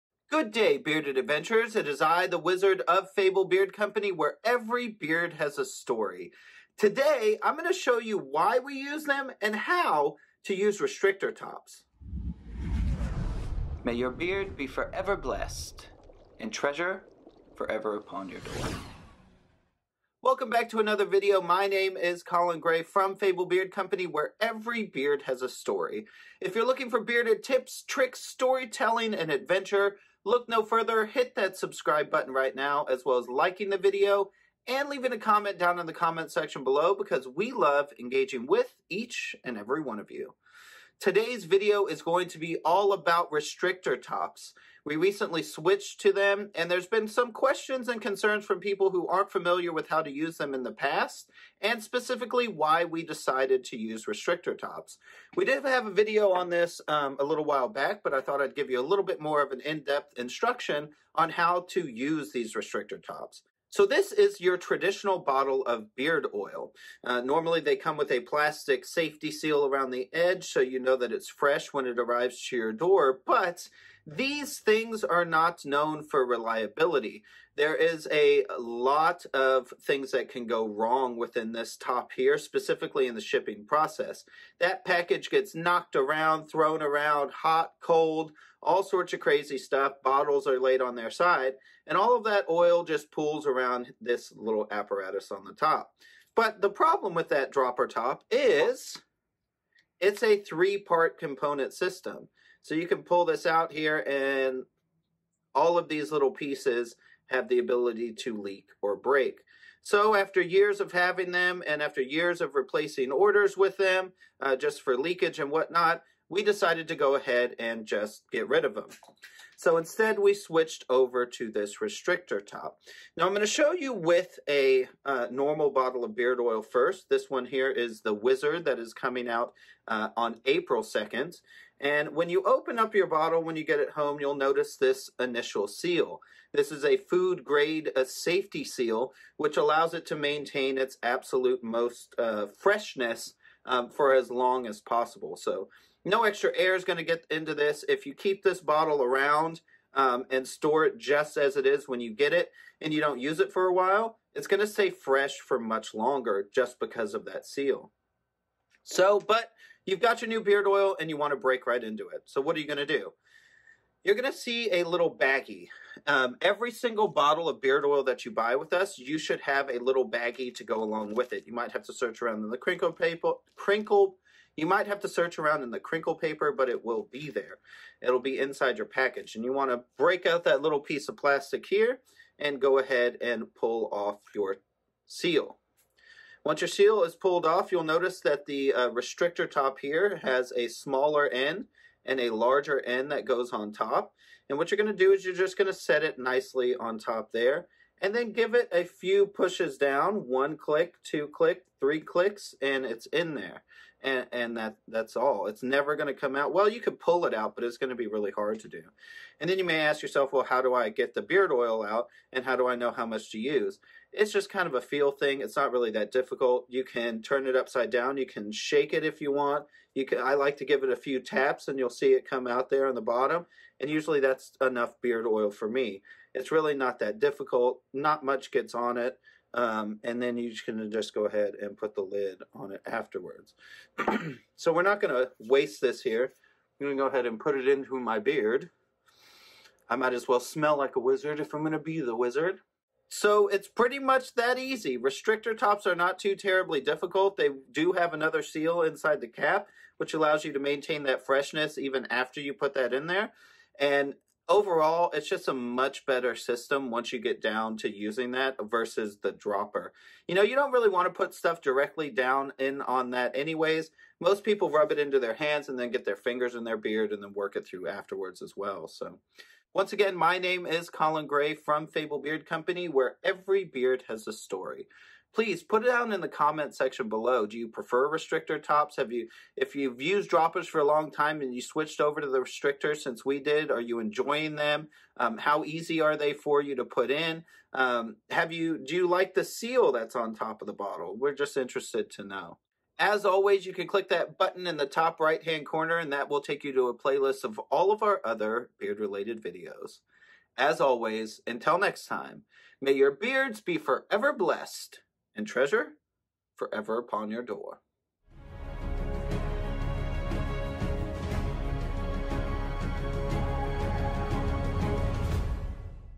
How To Use Restrictor Tops For Beard Oil
In today's video, we are going to discuss how to use restrictor tops for beard oils.